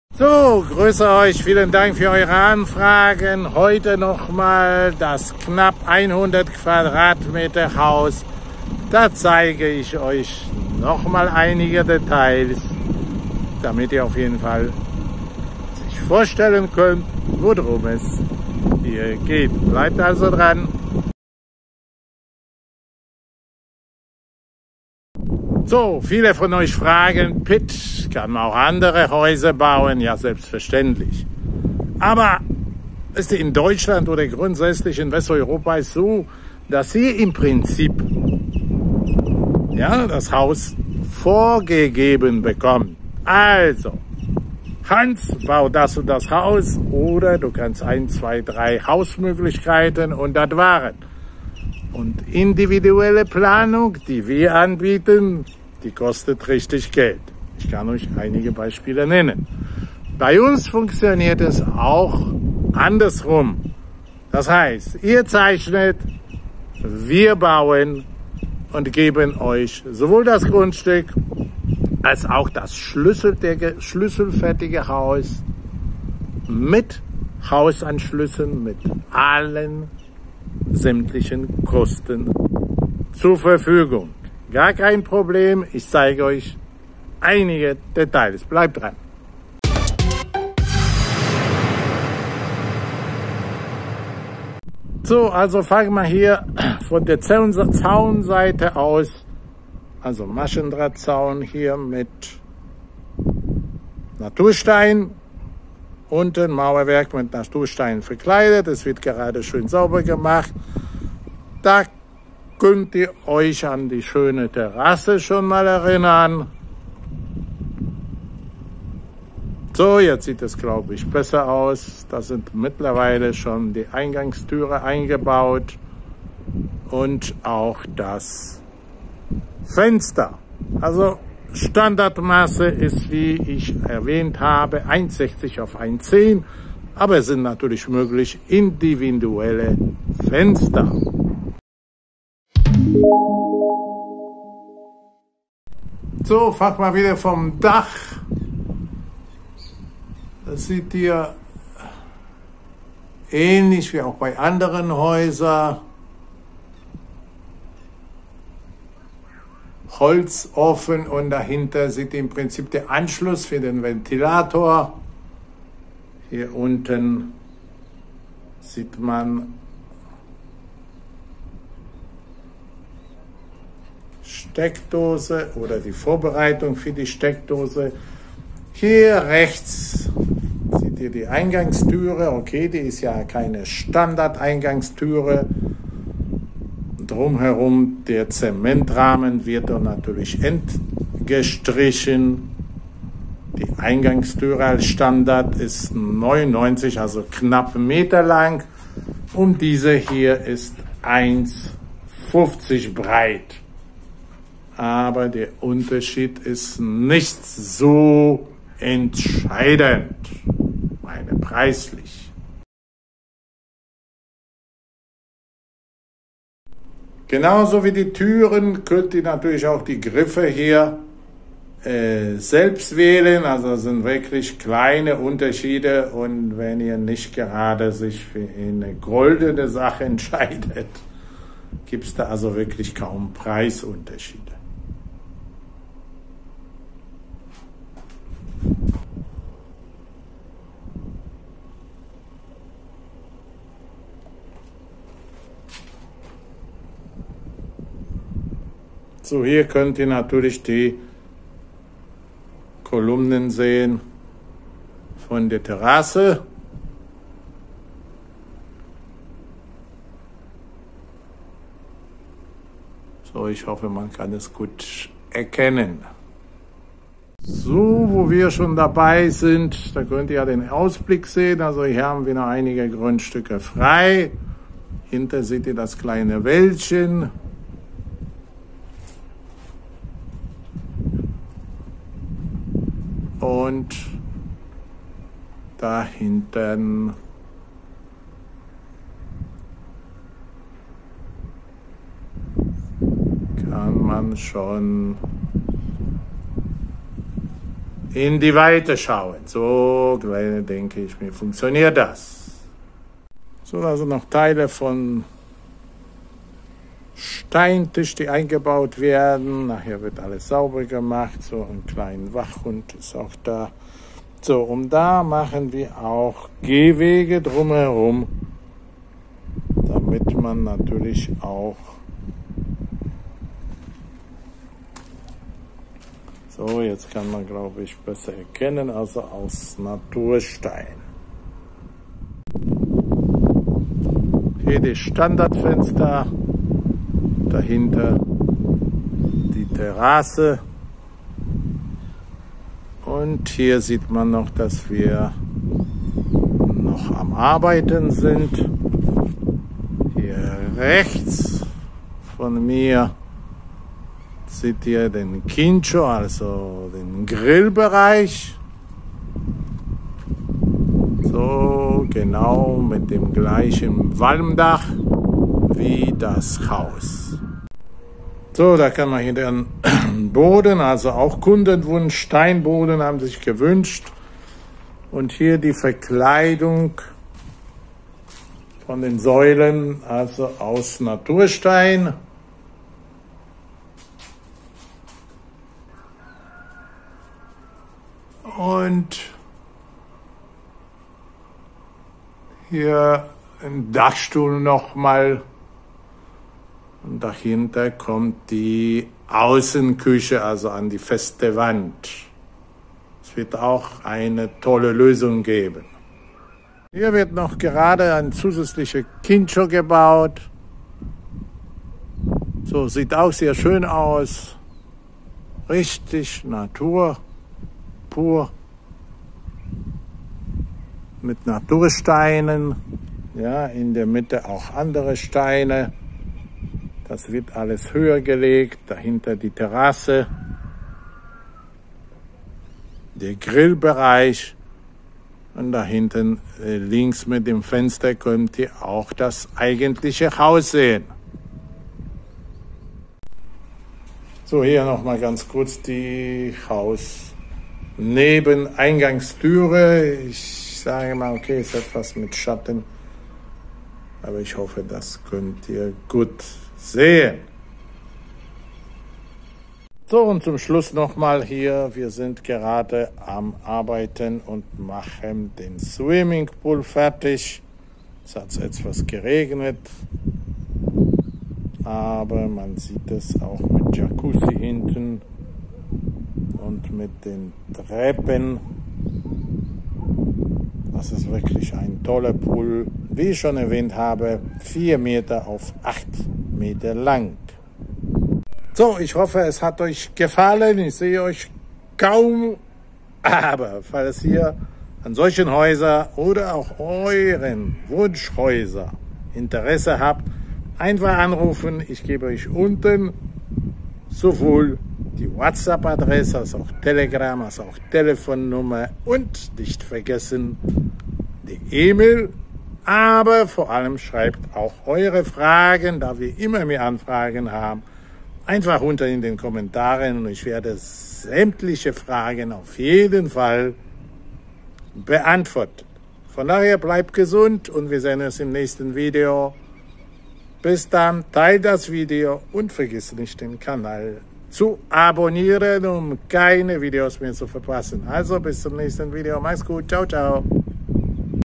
Günstiger Bungalow 100 qm Hausbauen mit 4 Zimmer, Bad Paraguay Live Haus kaufen
Haus bauen kosten: Da stellen viele die Frage eben nach den Kosten. Eine vernünftige Bauweise sollte schon sein, daher sollte man mit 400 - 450 Euro/m2 schlüsselfertig rechnen (Stand: März 2022). Das Haus ist dann schlüsselfertig! Dazu müsst ihr noch die Nebenkosten rechnen wie: Zaunbau, Außenanlagen, event. Swimmingpool. Grillbereich e.t.c und noch das Baugrundstück dazu.

Fragen bitte an:
stellen.